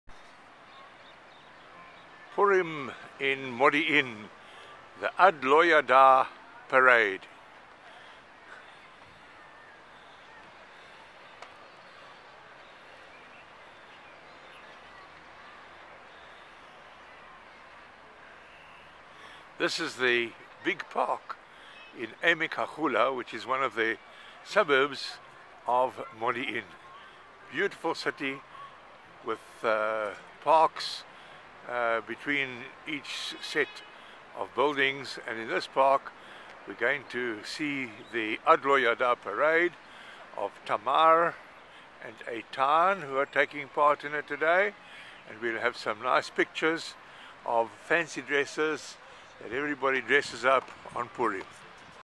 Adloyada in Modiin
Purim is always a great celebration. It's lots of fun because everyone gets dressed up in fancy dress and gives gifts to everyone.